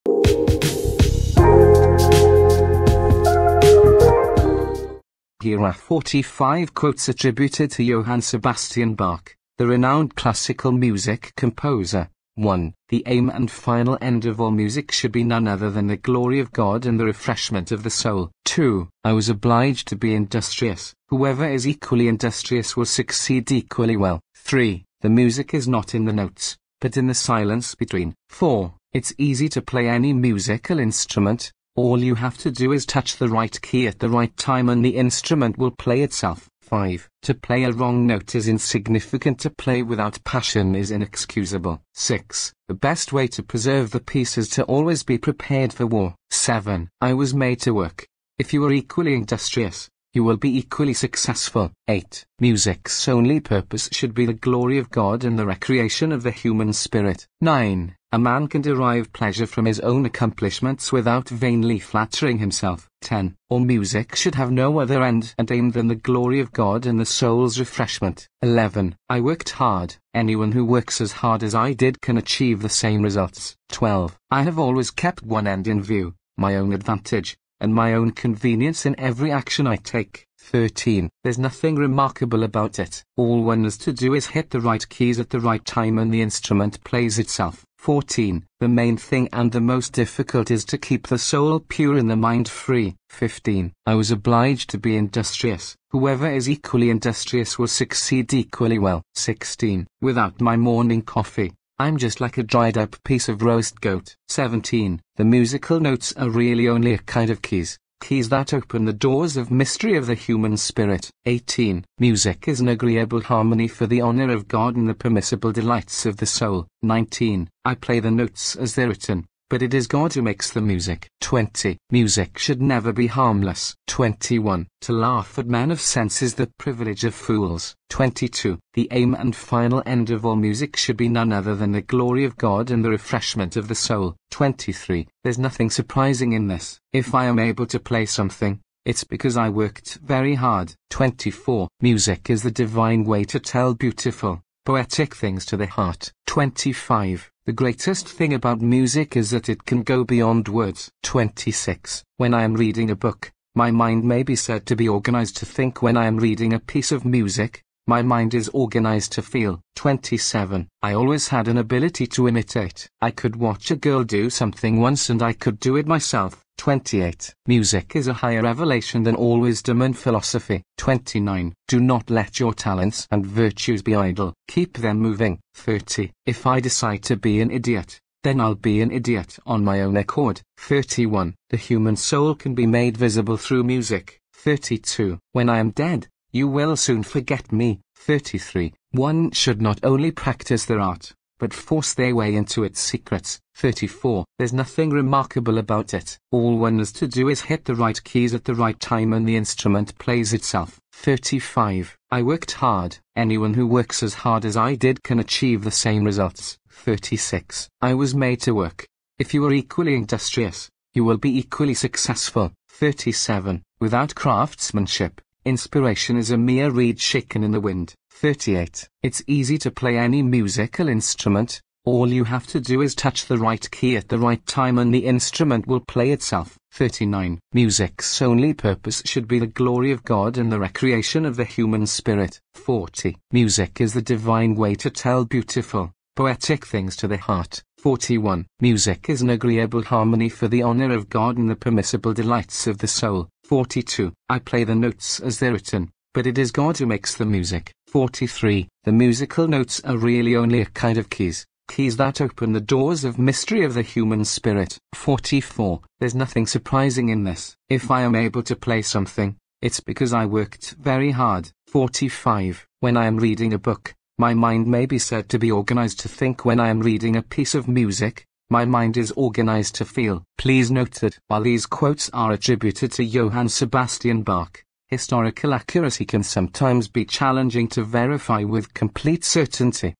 Timeless Wisdom: Inspiring Quotes by Johann Sebastian Bach
In this mesmerizing video, we bring you a collection of insightful and profound quotes by the legendary composer Johann Sebastian Bach. Immerse yourself in the wisdom and inspiration that emanates from his words, resonating through the corridors of time. As a master of both music and life, Bach's quotes offer glimpses into his creative process, his dedication to his craft, and his profound understanding of the human spirit. Whether you're a musician, an artist, or simply someone seeking inspiration, these quotes are sure to ignite your passion and awaken your inner creativity. Let the genius of Bach guide you on a journey of self-discovery and artistic exploration. 🎵✨

Also, check out the channel's podcast "The Sound Around Us!", wherever you listen to podcasts!